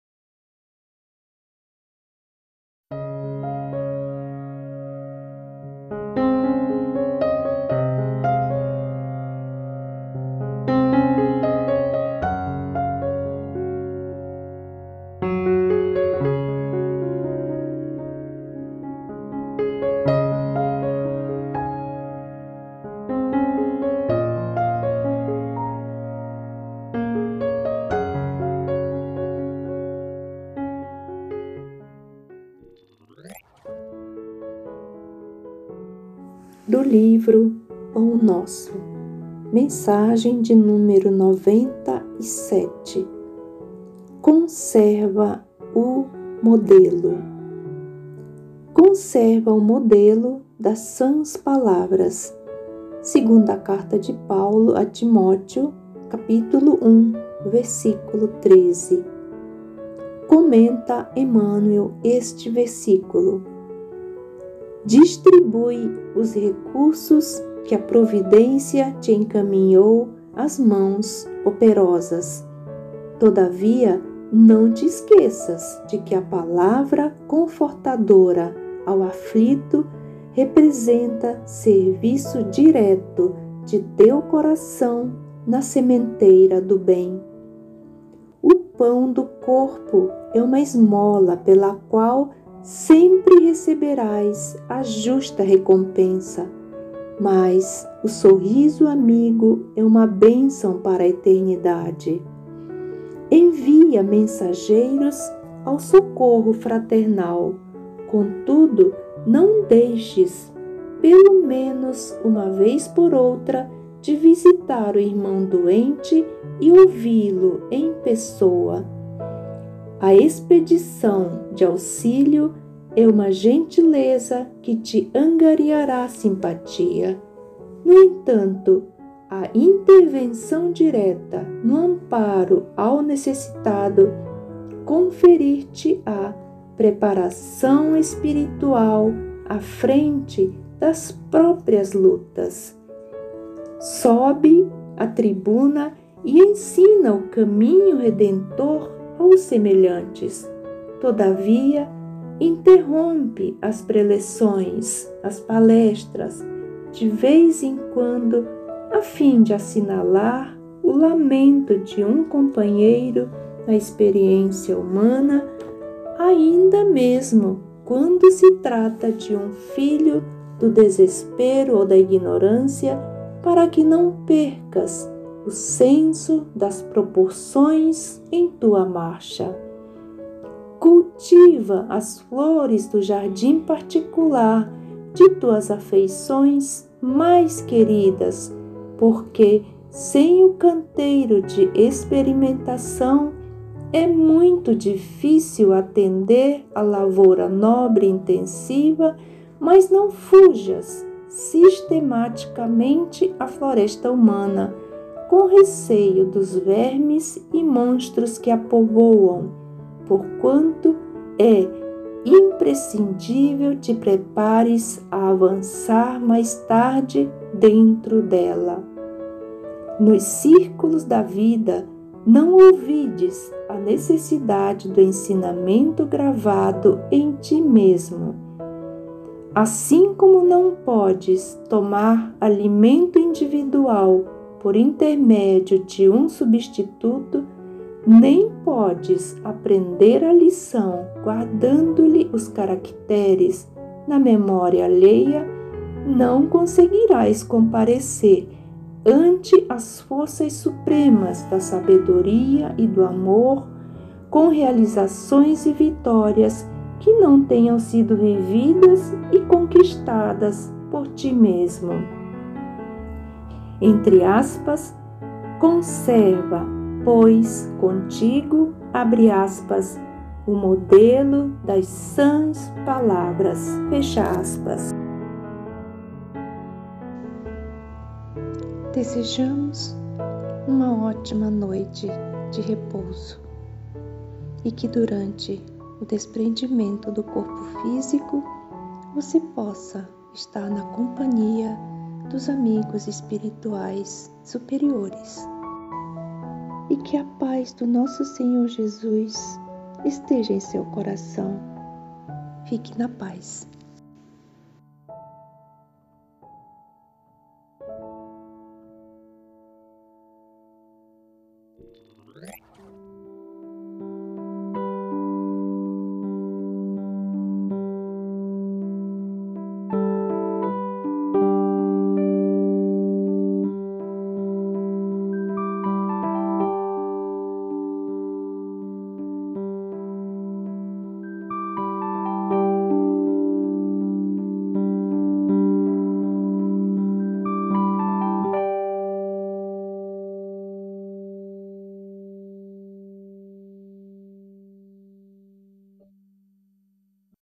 Leitura ao Anoitecer: Pão Nosso - Leitura 097: Conserva o modelo
Leitura realizada pelo Grupo EOS, em Manaus, do livro "Pão Nosso”, ditado pelo Espírito Emmanuel e psicografada por Francisco Cândido Xavier, em 1950.

Compõe a coleção "Fonte Viva" juntamente com os seguintes livros: Pão nosso; Vinha de luz; Fonte viva, todos eles da autoria espiritual de Emmanuel.  Neles, o autor comenta ensinamentos do Evangelho de forma original e atraente, ensinando-nos não apenas a compreender a doutrina cristã, mas a praticá-la em todos os momentos da vida.

Neste vídeo a Tia realiza a leitura da mensagem número 97, com o título "Conserva o modelo".

Que Jesus nos abençoe neste estudo!